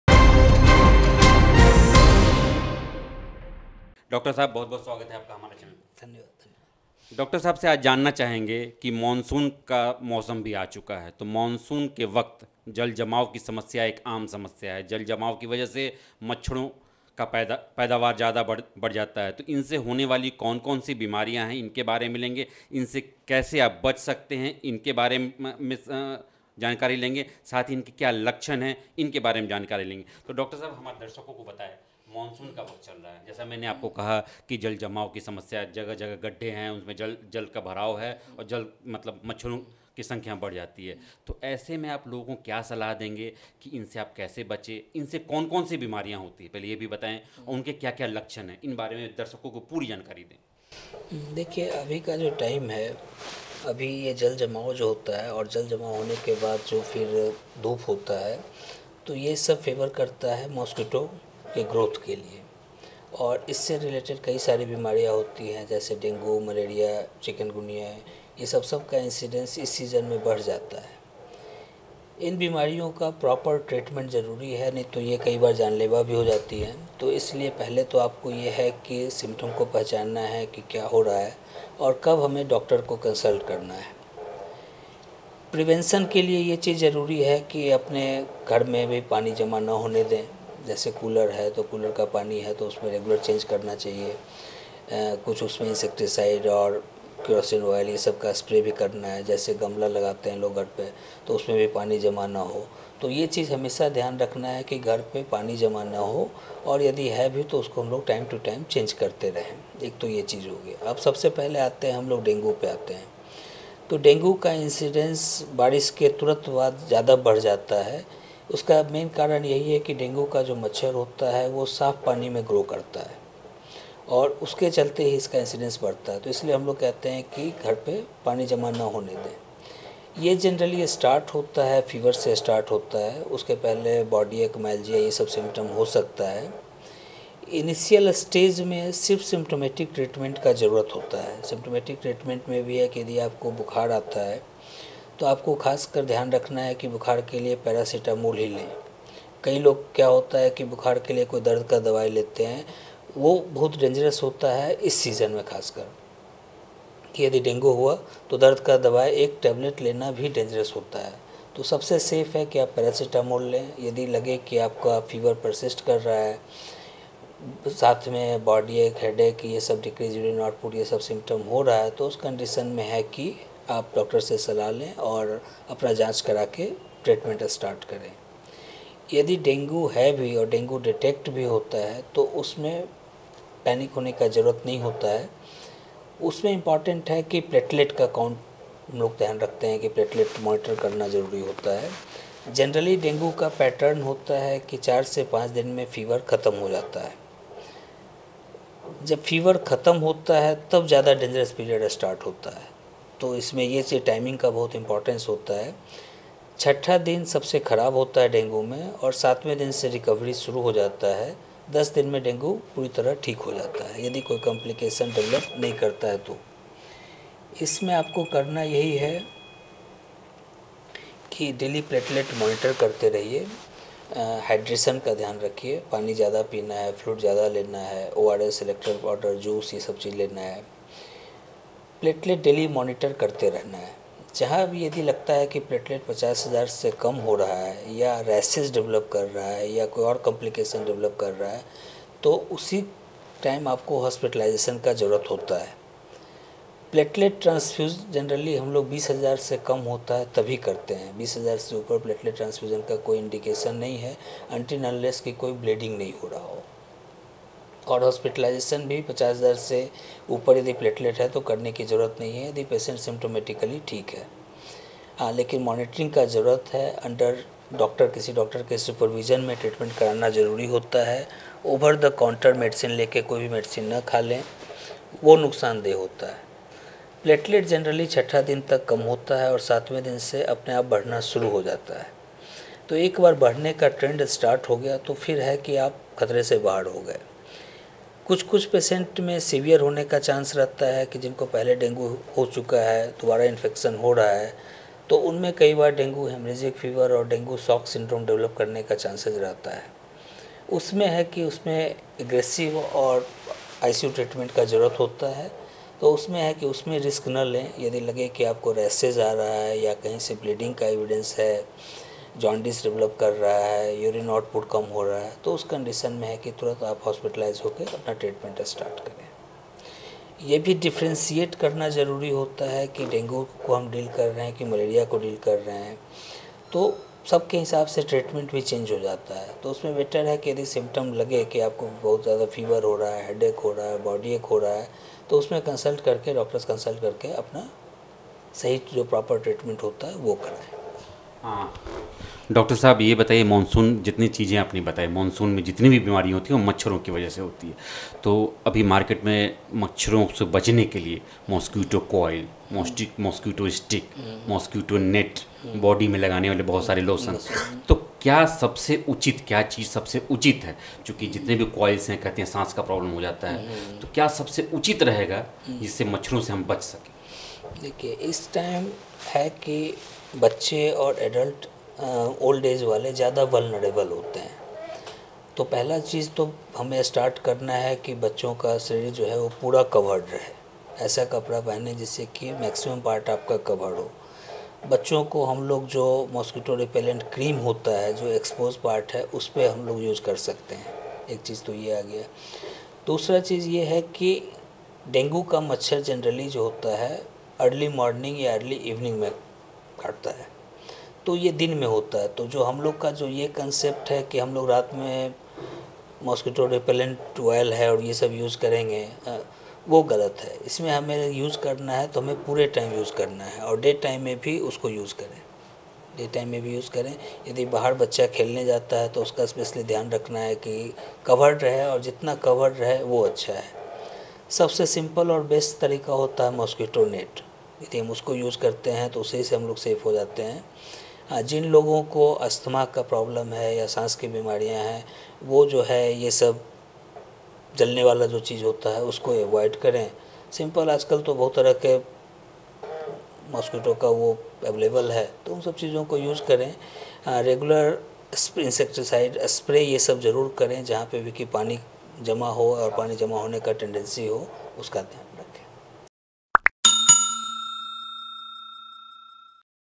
Monsoon में Mosquitoes से कैसे करें अपना बचाव? क्या है आसान तरीका, बता रहे हैं Dr. Mukesh Kumar
मानसून में मच्छरों से कैसे करें अपना बचाव? क्या है आसान तरीका, जानिए बता रहे हैं डॉ. मुकेश कुमार, देखिए वीडियो...